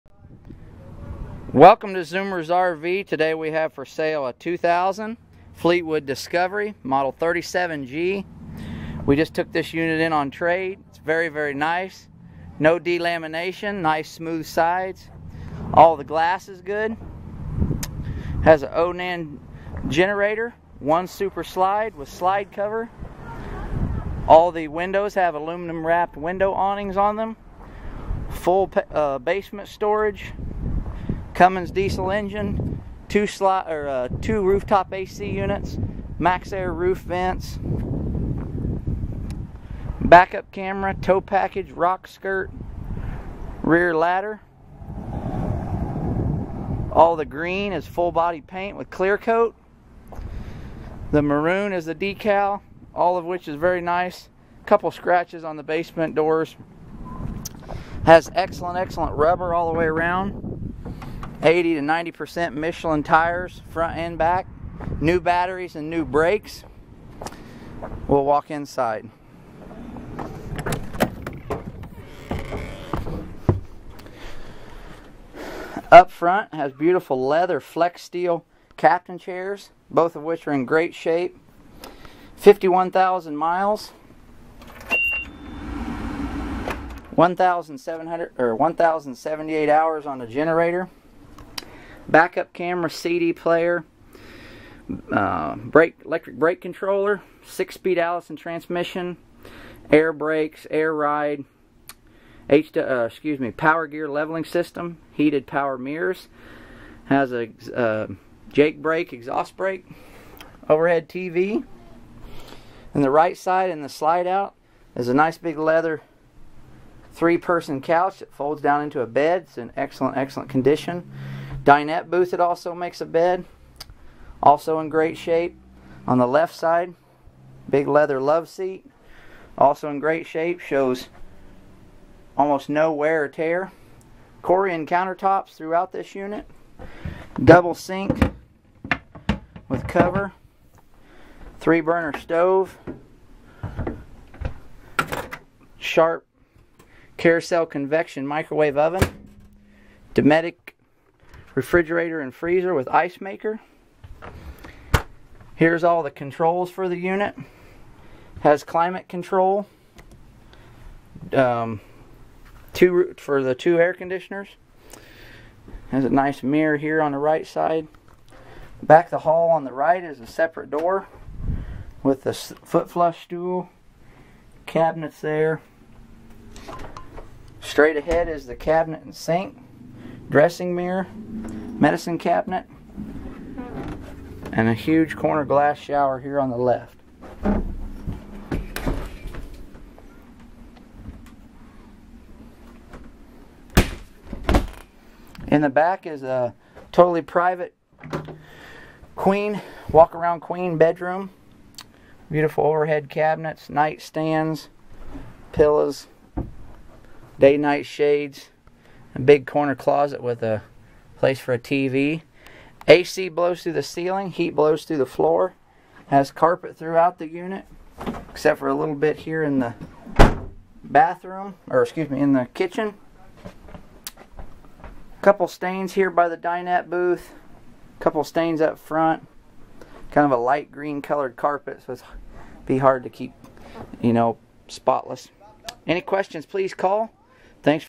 Very Nice 37' 2000 Fleetwood Discovery 37G 1-Slide, 51K Miles!!!
Very Nice 37' 2000 Fleetwood Discovery 37G 1-Slide, 51K Miles, Cummins Diesel, Allison 6 Speed, Jake Break, Air Brakes, 2 Roof AC's, All Leather, Onan Generator, NEW Tires, Batteries, and Brakes.. Just Traded It In on a New Unit!!  Very Well Maintained!!  And SPIT SHINED by the TWIN HELPERS!!!   Average Current NADA Book $48,210  ZOOMERS SUPER LOW $34,900 MONTHLY PAYMENT $276 NO MONEY DOWN with approved credit!!!